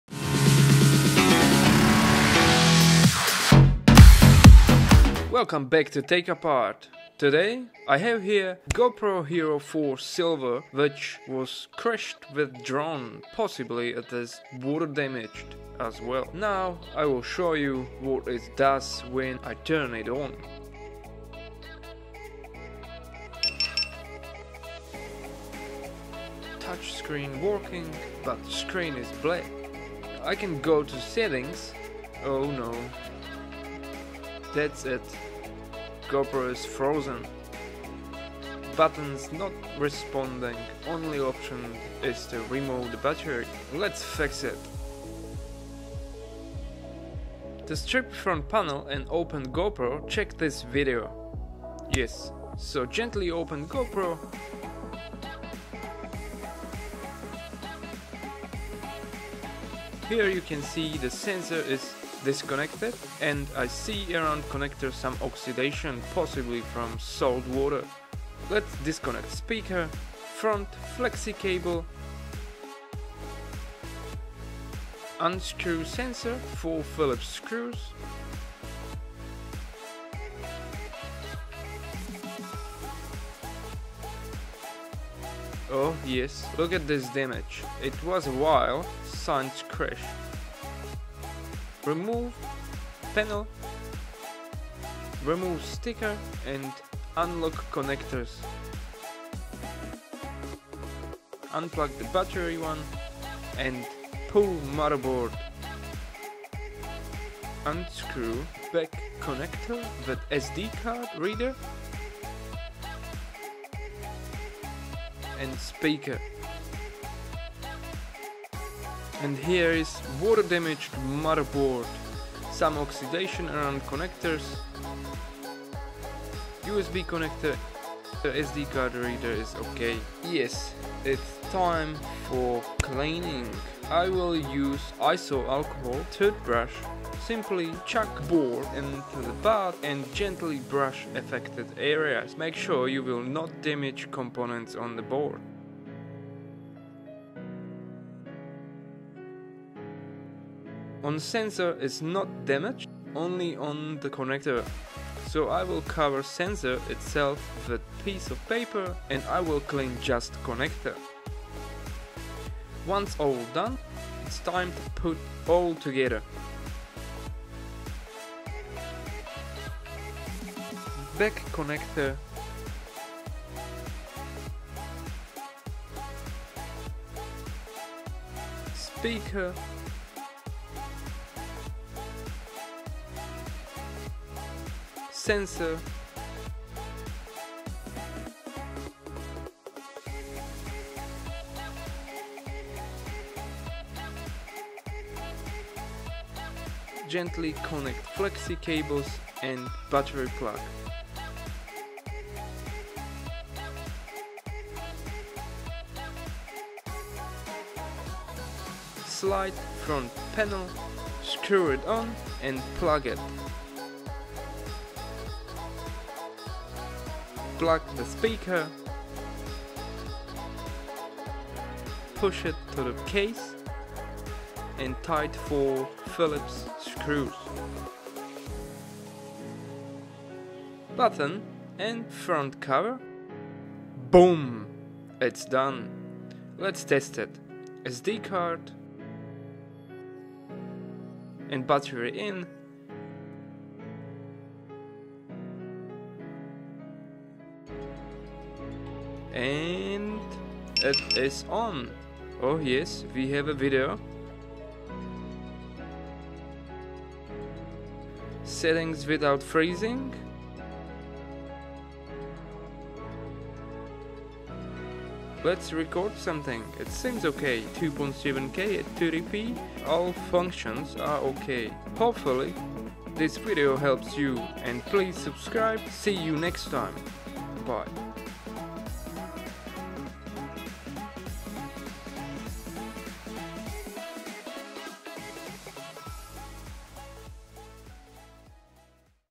Water Damaged GoPro Hero 4 Silver | How To Fix It | [4K]
While ago This GoPro Hero4 silver edition was crashed in to the water mounted on the drone, when it come to my hands GoPro had few issues including not showing pictures and it always freezes and turn it self to a brick!

and then I find disconnected sensor inside of cmera and some oxidation on motherboard around the connectors.
I was lucky because after cleaning and reassembly the GoPro is in working order.

GoPro Motherboard :

Opening Tools:

GoPro tighten :